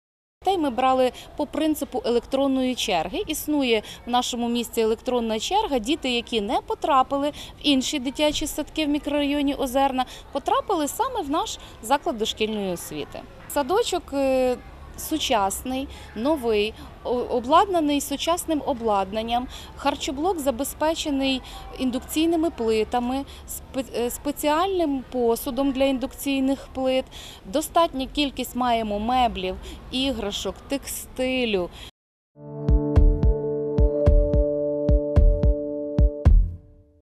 350 дітей пішли у новий садочок, який відкрили у мікрорайоні Озерна
350 дітей вступили до Хмельницького дошкільного навчального закладу №9 «Кобзарик», який відкрили сьогодні у мікрорайоні Озерна. Завідуюча цього садочку Людмила Королишина розповіла, сформовано 11 груп, в кожній в середньому по 30 дітей. Додає, це перший дитячий садочок, за 25 років, який відкрили у цьому мікрорайоні міста.